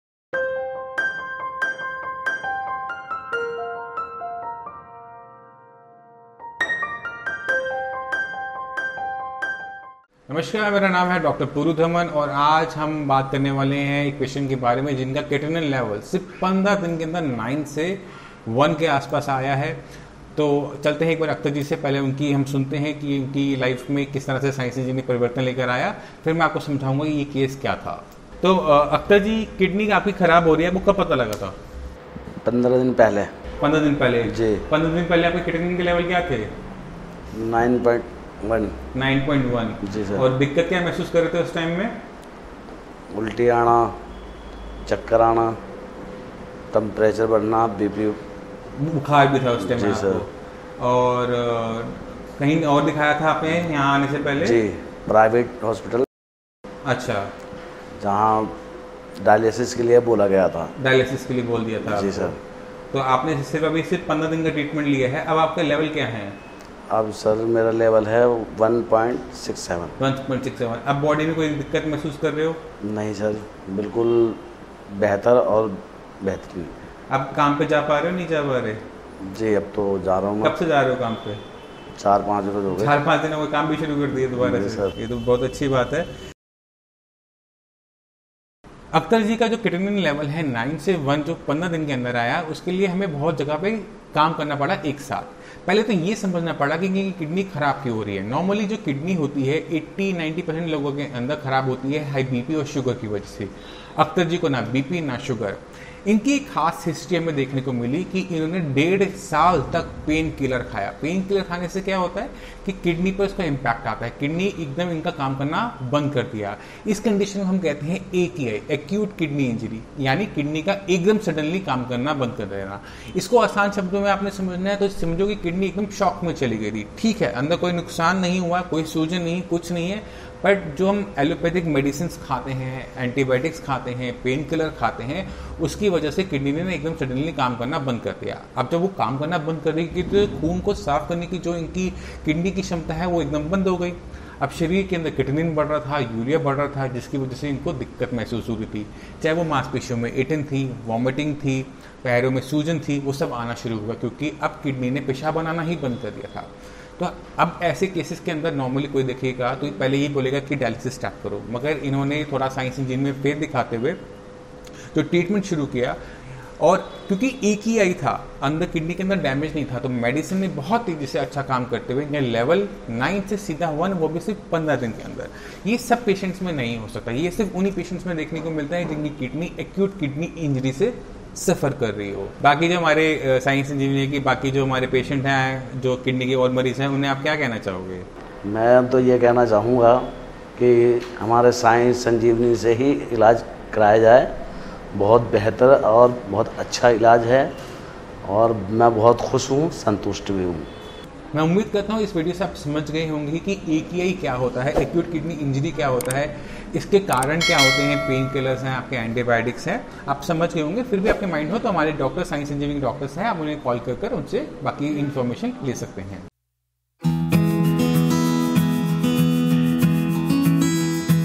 Creatinine Reduced From 9.1 to 1.67 in 15 days
Tips to Reduce Creatinine | Dr Puru Dhawan

In this video, Dr. Puru Dhawan talks with a patient treated at Sai Sanjivani.

Mr. Akhtar came to know about his deteriorating kidney health 15 days before and he directly came to Sai Sanjivani. He was suffering from kidney disease due to painkillers as he consumed painkillers for more than a year. Earlier he was facing vomiting, body ache, and other symptoms. Only in 15days, his creatinine level decreased from 9.1 to 1.67

𝐁𝐨𝐨𝐤 𝐀𝐩𝐩𝐨𝐢𝐧𝐭𝐦𝐞𝐧𝐭 - 𝟵𝟴𝟭𝟭𝟳𝟰𝟰𝟵𝟵𝟵 (𝟭𝟭𝗔𝗠 𝘁𝗼 𝟲 𝗣𝗠)
𝐀𝐝𝐝𝐫𝐞𝐬𝐬: 𝐁-𝟗𝟐, 𝐒𝐮𝐬𝐡𝐚𝐧𝐭 𝐋𝐨𝐤, 𝐏𝐡𝐚𝐬𝐞 𝟏, 𝐆𝐮𝐫𝐮𝐠𝐫𝐚𝐦, 𝐇𝐚𝐫𝐲𝐚𝐧𝐚 - 𝟏𝟐𝟐𝟎𝟎𝟗 (𝐍𝐞𝐚𝐫 𝐇𝐮𝐝𝐚 𝐂𝐢𝐭𝐲 𝐌𝐞𝐭𝐫𝐨 𝐒𝐭𝐚𝐭𝐢𝐨𝐧)